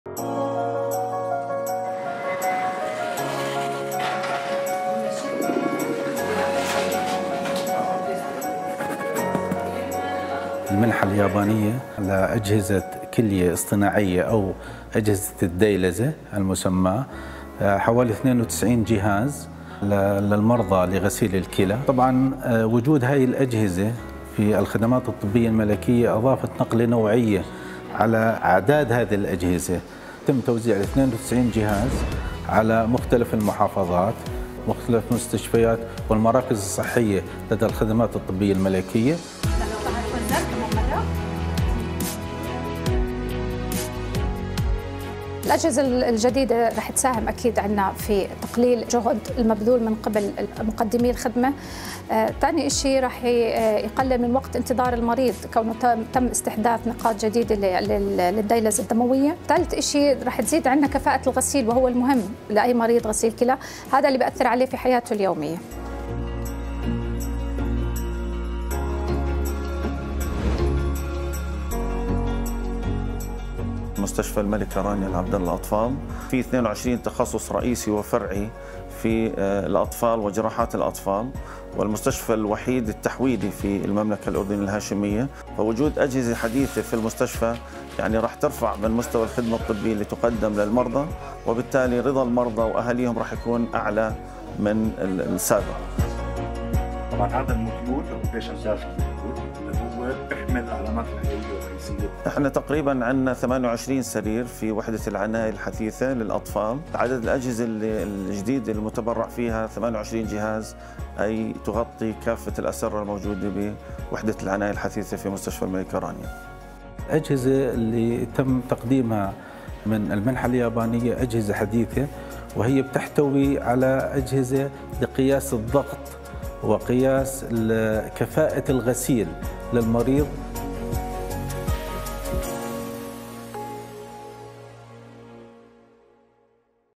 دعم الخدمات الطبية الملكية في الأردن
تساعد المعدات الطبية الجديدة التي قام مكتب الأمم المتحدة لخدمات المشاريع بتوفيرها بتمويل من اليابان على تحسين الرعاية الصحية في المنشآت التي تديرها الخدمات الطبية الملكية في الأردن

شاهد الفيديو لمعرفة المزيد